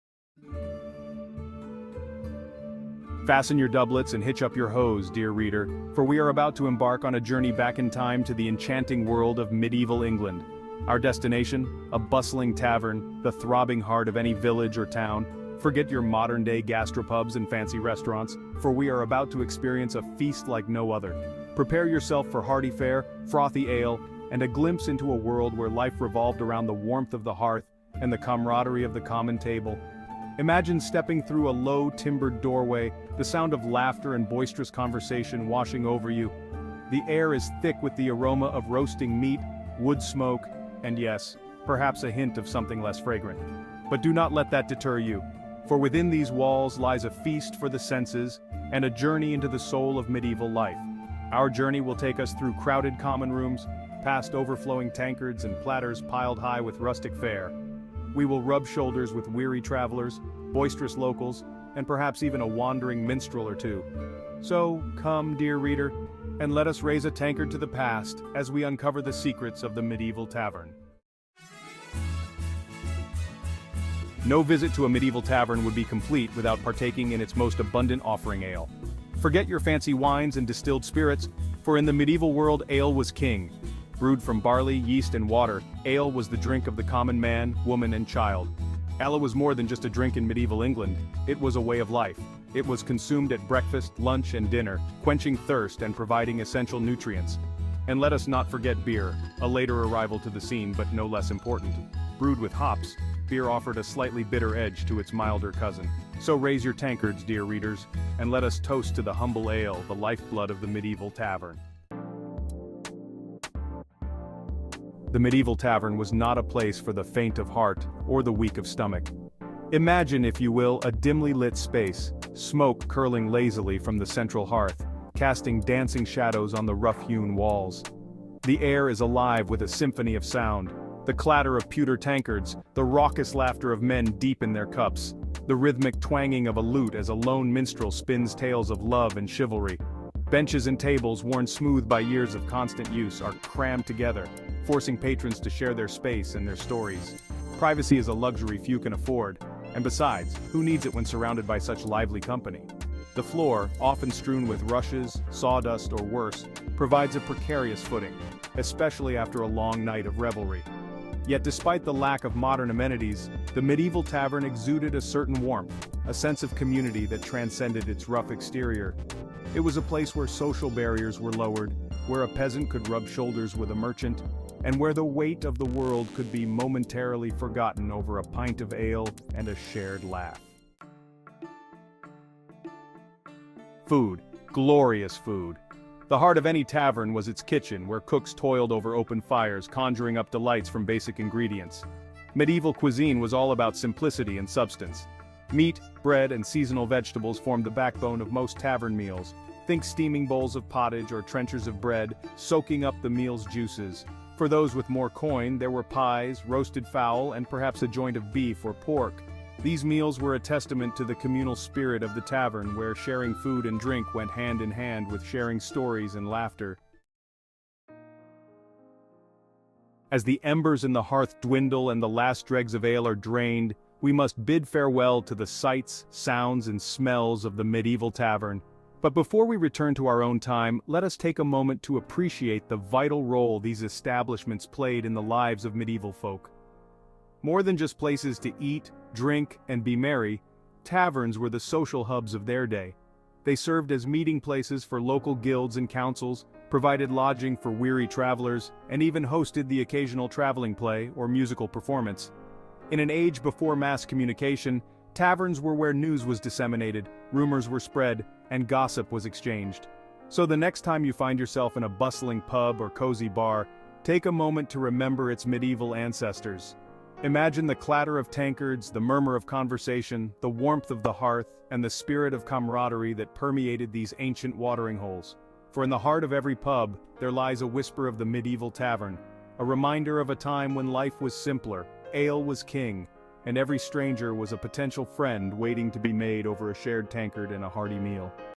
A Meal at a Medieval Tavern
Step back in time and immerse yourself in the lively atmosphere of a medieval tavern, the true heart of any village or town. Discover the essential role of frothy ale, not just as a daily beverage but as a social lubricant and a symbol of hospitality, safer and more nutritious than water. Experience the communal dining experience, where simple yet hearty fare like pottage and roasted meats bring communities together. Hear the clatter of tankards, the bursts of laughter, and the strumming of lute strings as we explore the sights, sounds, and colorful characters that filled these vital social hubs. Learn how taverns weren't just for dining; they were crucial for lodging travelers, hosting entertainment, and even spreading news in an era long before modern communications. Dive into the bustling life of a medieval tavern, where every corner has a story.

CHAPTERS:
00:00:00
Welcome to the Medieval Feast
00:01:10
A Toast to Ale and Beer
00:01:51
The Sights and Sounds of a Medieval Tavern
00:03:00
More Than Just a Meal
00:03:36
Taverns - The Heart of Medieval Life

Share your thoughts and questions in the comments below. What other historical culinary journeys would you like us to explore?